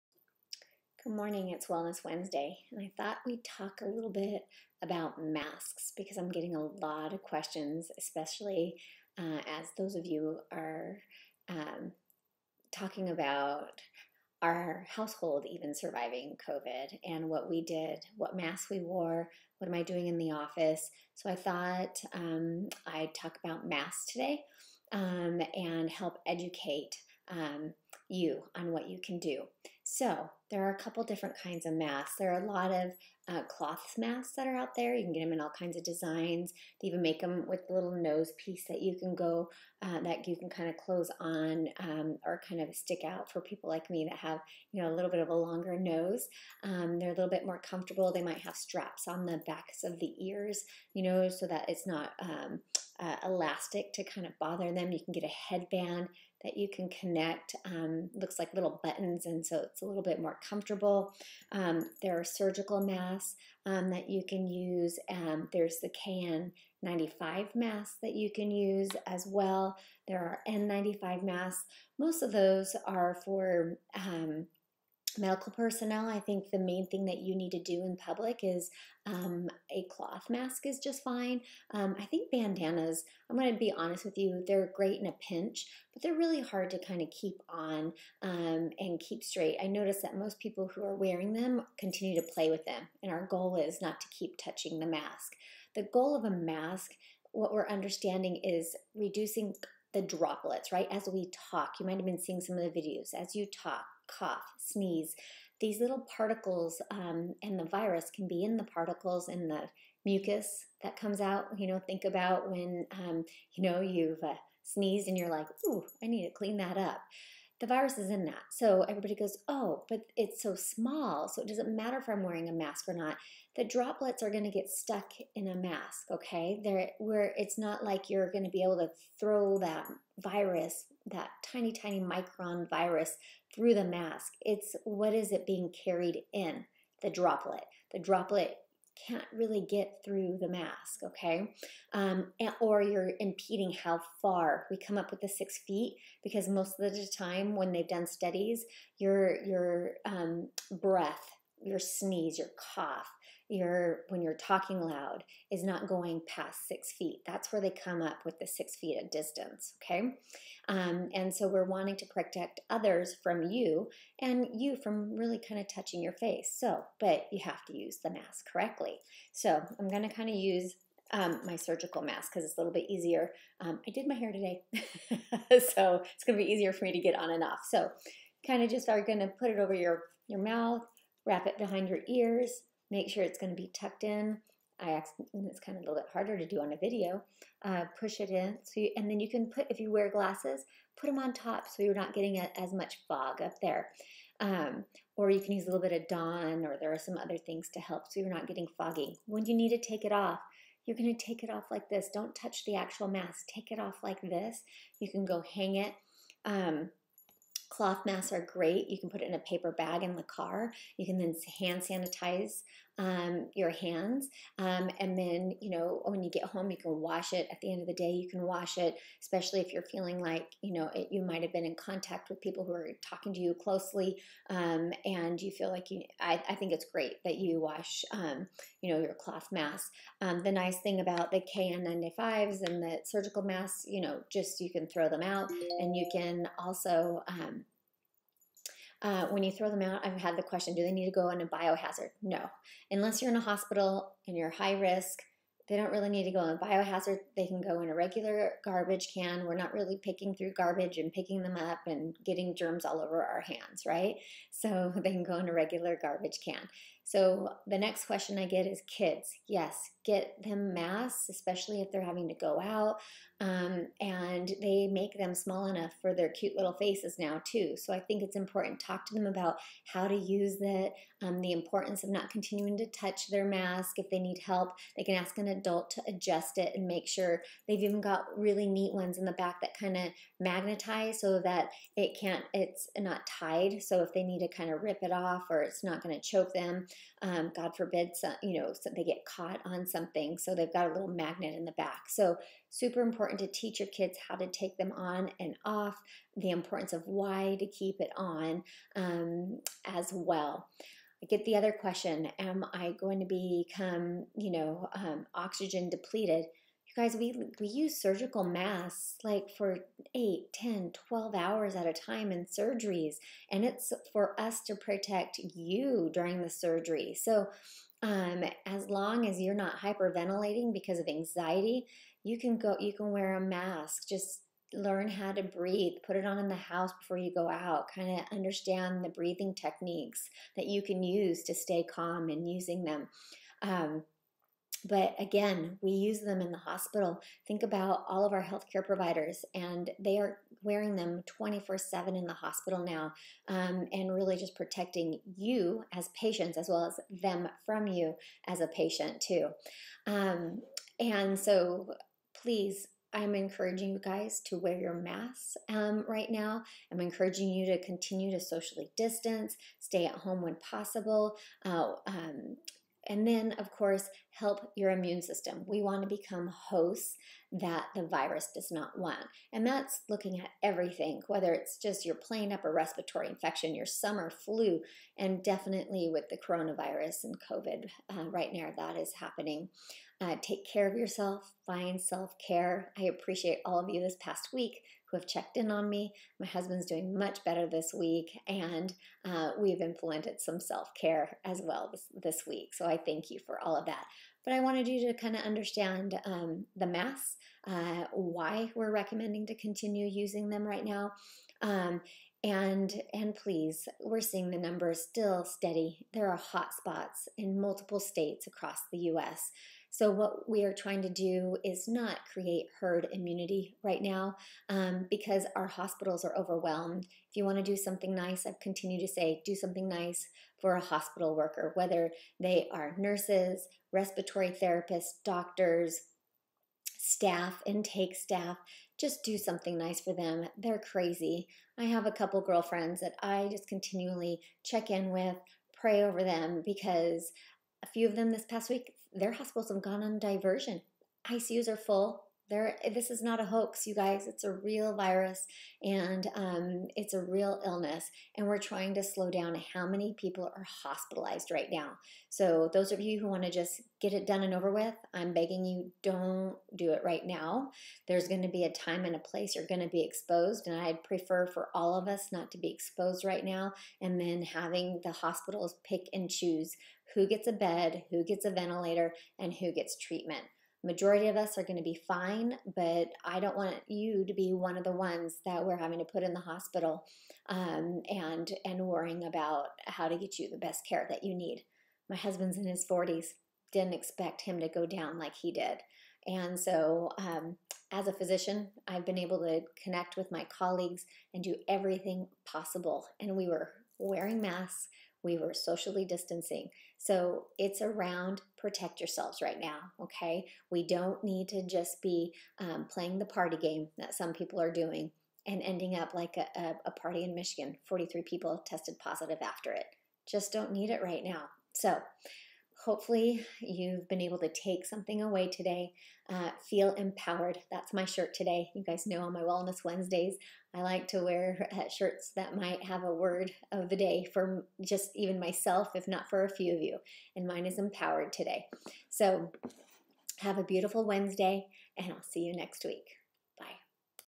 Wellness Wednesday - AZ COVID cases are on the rise. Masks are mandatory; how to wear one and why.
Lean in and listen as Dr. Rodarte demonstrates how to wear a mask and more importantly, why - don't argue with science, protect yourself and those around you buy wearing a mask.  And don't forget the wee ones!  They need one too!